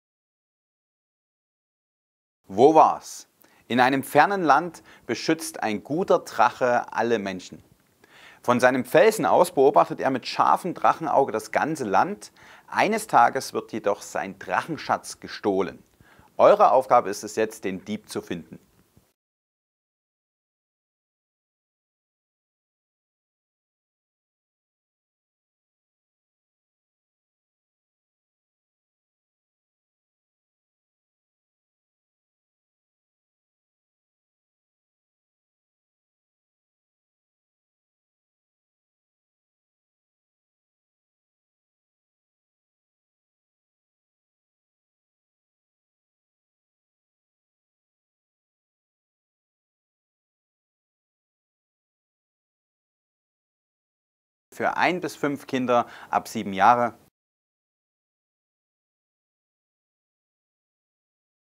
Ravensburger Wo war's?
Kinderspiel Wo war's? von Ravensburger

Helft dem Drachen, den Dieb seines Schatzes zu stellen!
Der Drache sucht aus der Luft und ruft den Spielern zu, wo sich der Langfinger gerade aufhält. Durch genaues Zuhören und cleveres Kombinieren der Geräusche bleiben die Verfolger dem Dieb dicht auf den Fersen. Wer schafft es, den Dieb zu finden?
Die Elektronik sorgt dafür, dass jedes Spiel anders verläuft und aufs Neue spannend ist.